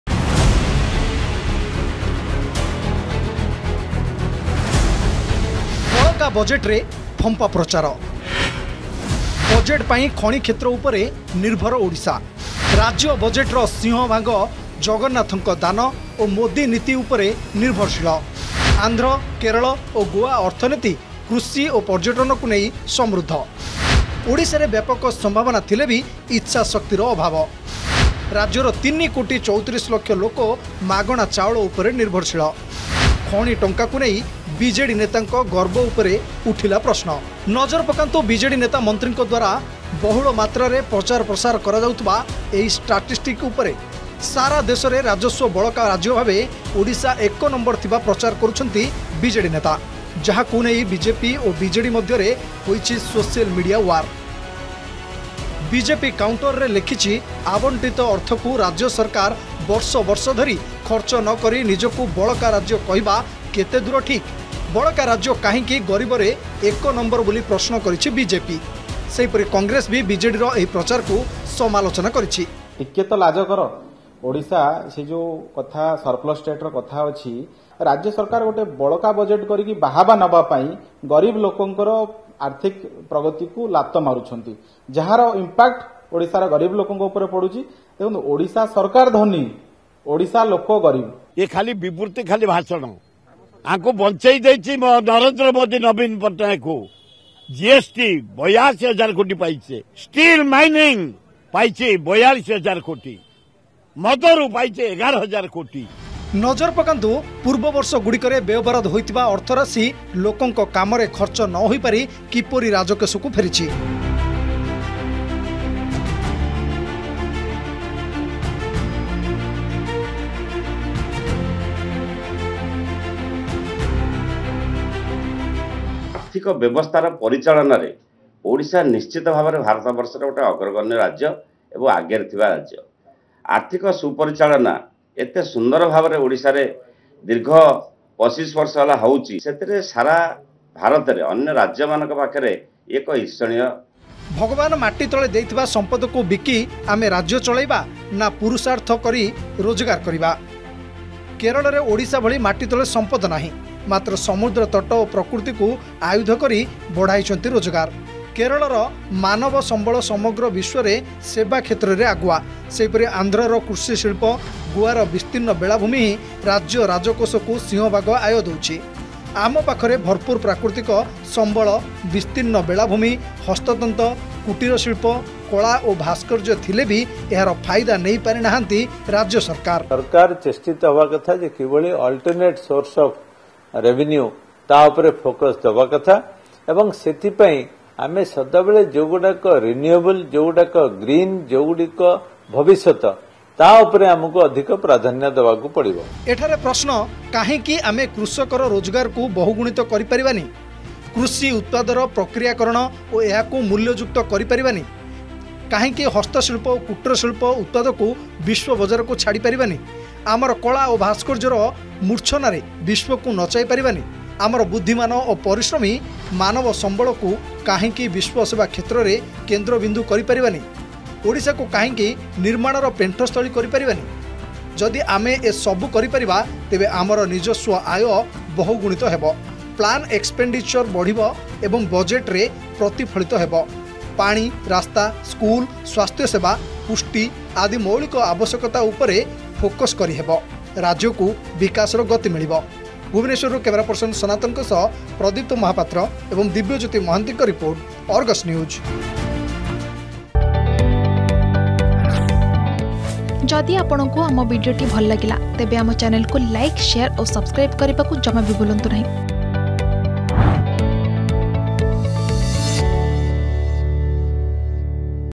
ବିଜେଡି ସରକାରର ବଳକା ବଜେଟର ଫମ୍ପା ପ୍ରଚାର || Budget || Odisha
ନଜର ପକାନ୍ତୁ ବିଜେଡି ନେତା ମନ୍ତ୍ରୀଙ୍କ ଦ୍ବାରା ବହୁଳ ମାତ୍ରାରେ ପ୍ରଚାର କରାଯାଉଥିବା ଏହି ଷ୍ଟାଟିଷ୍ଟିକ୍ ଉପରେ । ସାରା ଦେଶରେ ରାଜସ୍ବ ବଳକା ରାଜ୍ୟ ଭାବେ ଓଡିଶା ଏକ ନଂ ଥିବା ପ୍ରଚାର କରୁଛନ୍ତି ବିଜେଡି ନେତା । ଯାହାକୁ ନେଇ ବିଜେପି ଓ ବିଜେଡି ମଧ୍ୟରେ ହୋଇଛି ସୋଶିଆଲ ମିଡିଆ ଓ୍ବାର ।

Janata Mood || ରେମୁଣା ପାଇଁ କିଏ କିଏ ଆଶାୟୀ ପ୍ରାର୍ଥୀ